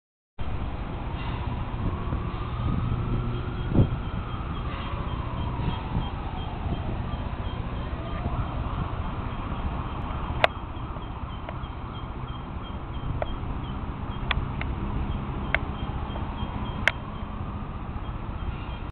Old Gosford quarry
Panorama of the site. It's got a construction sign up on it. Won't be long before there's some whacking block of flats there.